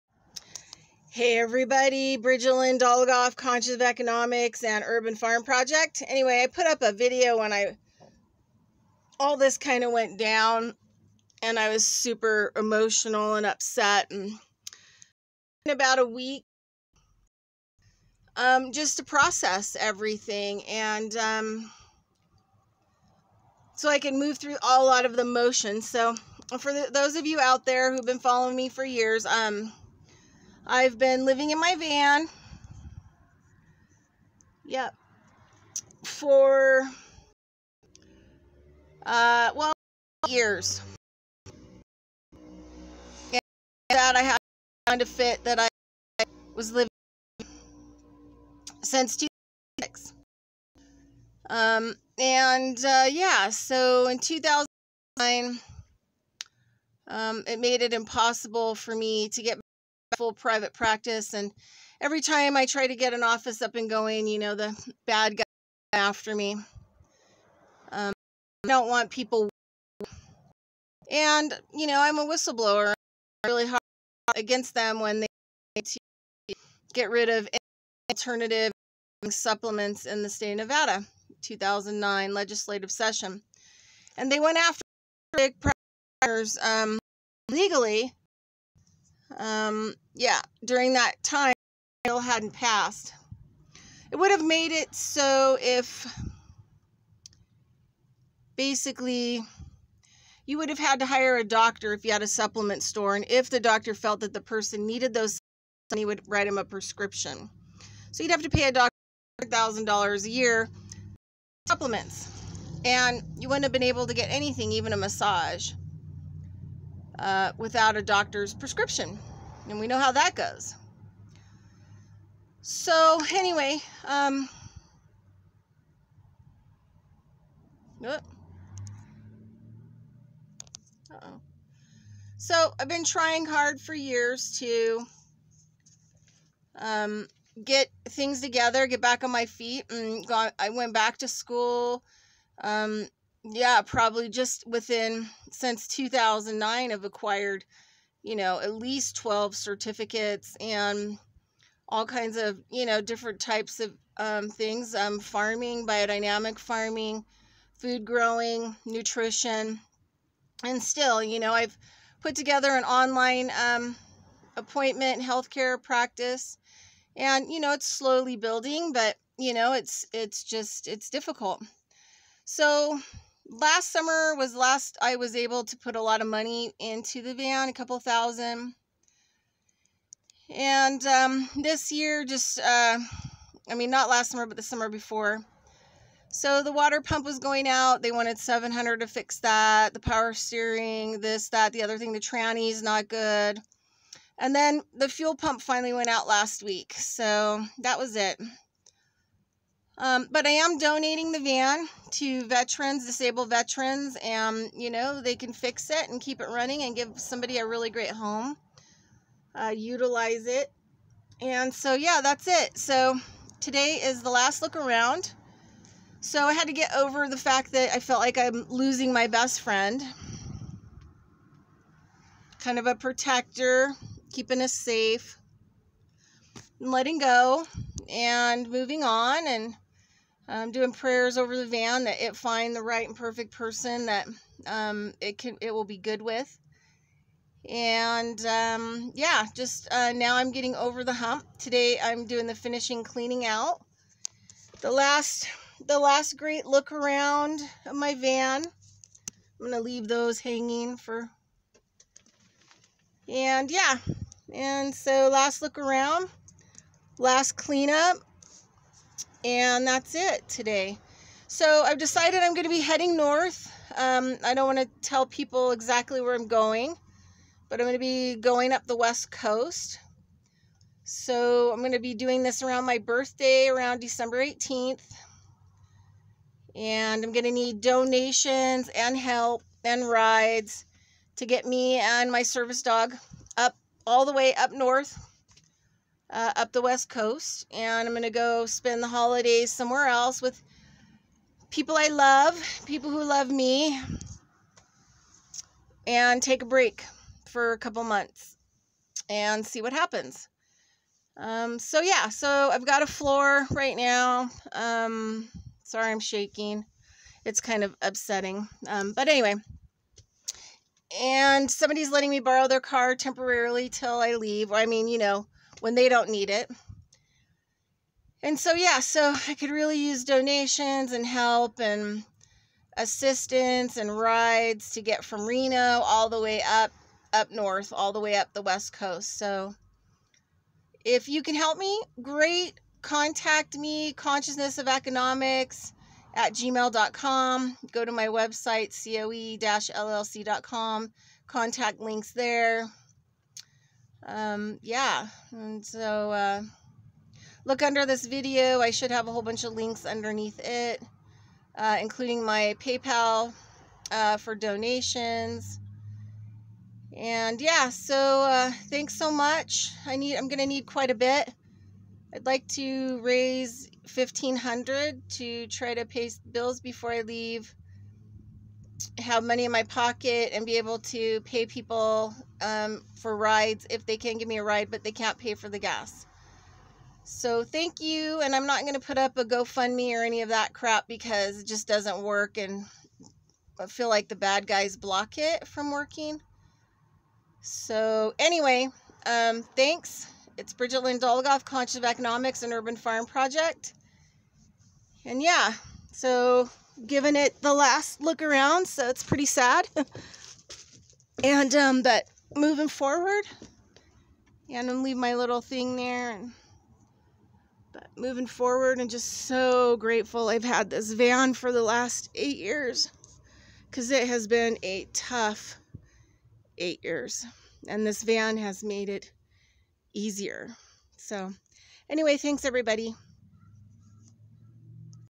My white Buffalo being towed away & i need help to go North for Holidays
Today I finished cleaning out my home for over 8 years. It was a really sad day, it felt like I lost my best friend. White Buffalo has kept me safe, out of storms and too many ceremonies to count. Tomorrow it gets towed away to a Veterans charity! I said goodbye, cried and left her with prayers of the future and safe passage and being the very best pony anyone could have! Took me a week of all over the map emotions: fear, terror, relief, letting go... and now on the other side of it and read to move forward. I have friends up North who are waiting for me to get up there for the holidays. I would like to start my travels around December 17, the day before my birthday. I will need rides from Reno, Nevada for me,  a few things and my service dog. I want to leave some information off of here like exactly where I am headed. I will be traveling up the west coast of the USA. I am also in need of funds since I no longer have a home and my home and wheels are heading on to the next journey. I have a temporary floor and I need funds to pay bills before I leave and to hit the road on foot. Everything else is in the video, thanks, new starts and I need a break. I cannot make enough money in Reno to get a roof, the cost of living is far more than I can pay. Time to move on, and see what else I can get myself into. If you can help with anything, I would be so grateful. Thank you. You can use the pay pal link, I would like to raise 1500 USD before I leave or I may not be able to leave. Rides need those and I can link them together from Reno to where I am headed and I especially need this help. I am will need a few stay overs and the last bit of a push at the end to get the last part completed. I have no family they have all died. I look at this as another vision quest spirit has got me, though I am a bit scared, I just keep on praying my way through and spirit has never failed me, the spirit has broken me before, but has never failed me. Peace out and in.